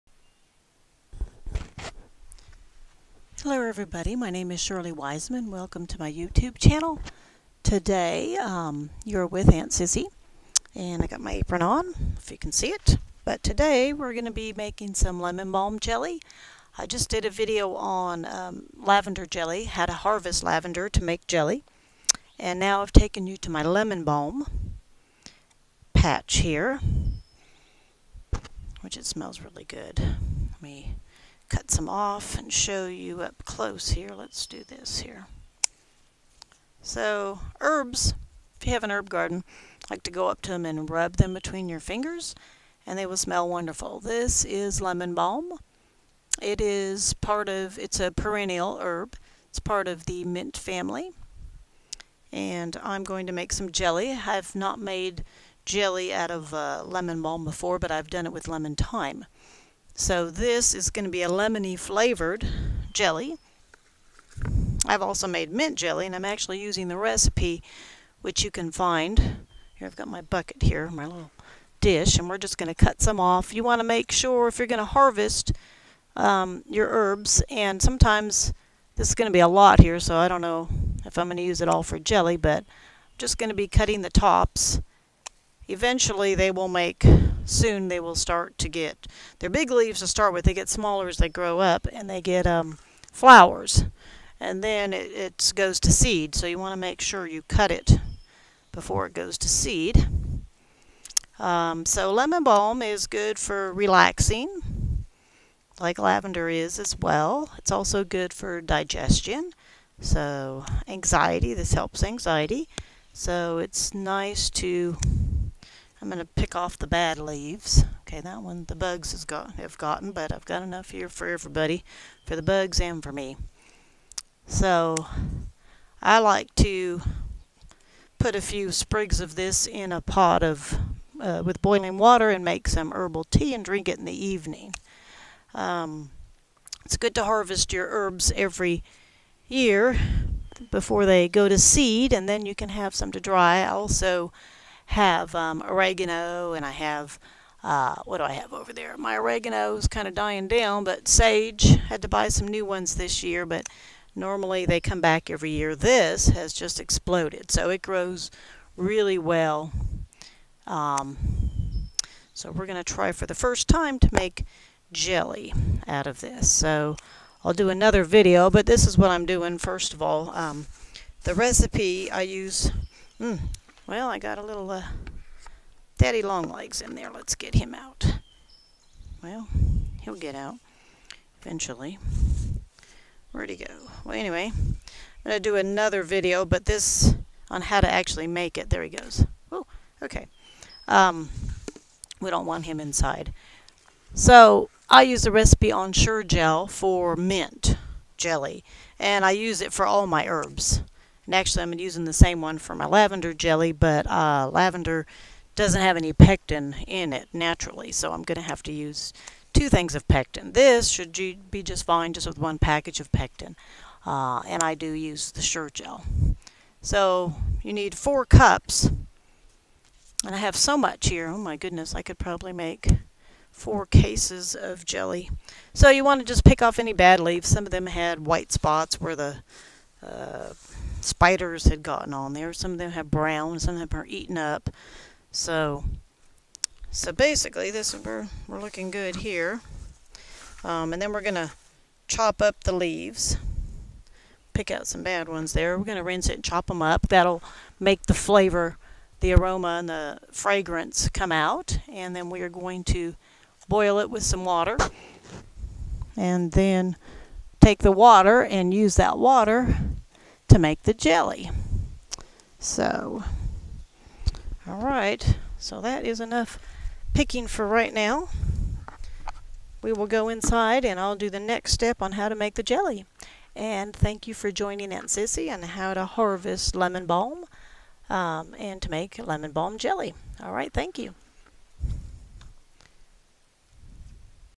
Harvesting Lemon Balm
Harvesting Lemon Balm to make Lemon Balm Jelly